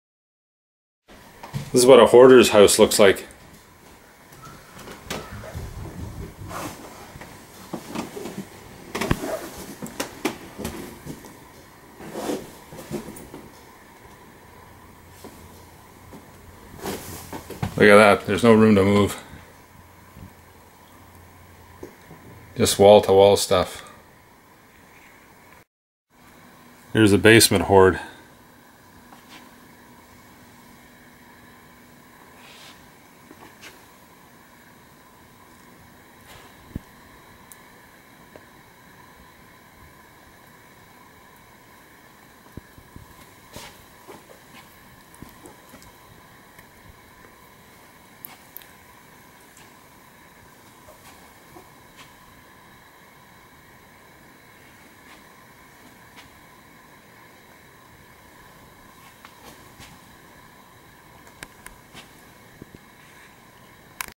House Mess During Renovation
We had to store furniture and household articles elsewhere while the living and ding rooms floors were being refinished.